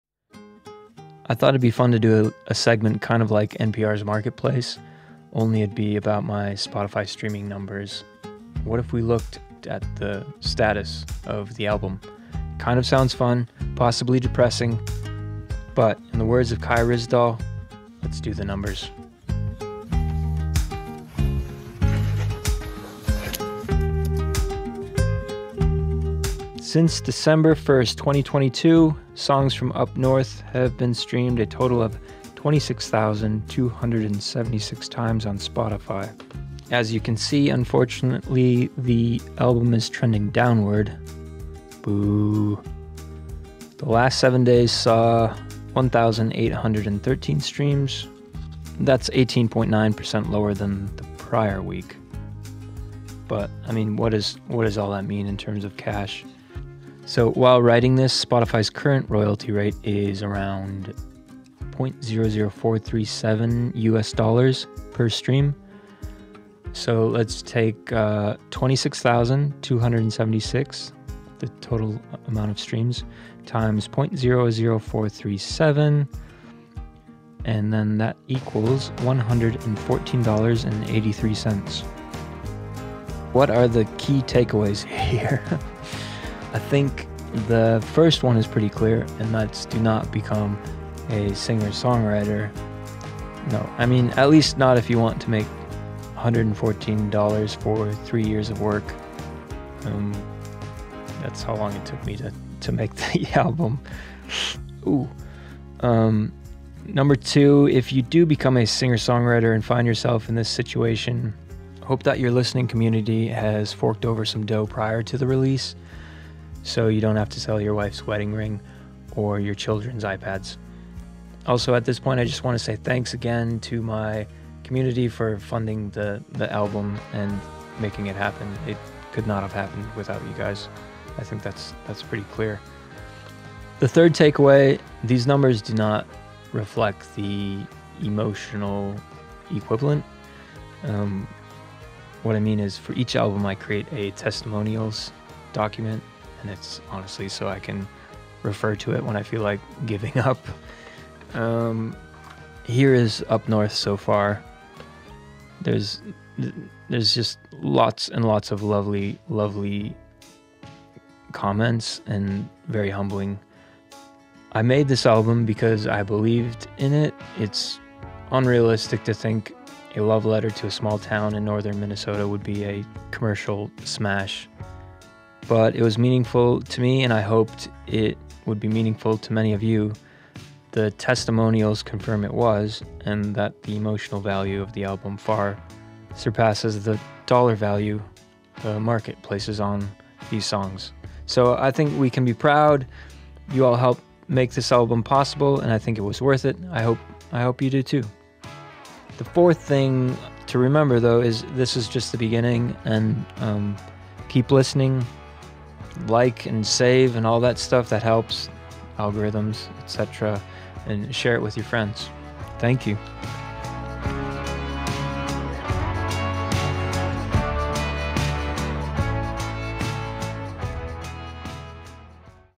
How Much Do I Make on Spotify?
to get the e-mail. Let's go into my Spotify dashboard together and pull back the curtain on my latest album. I think there are four key takeaways.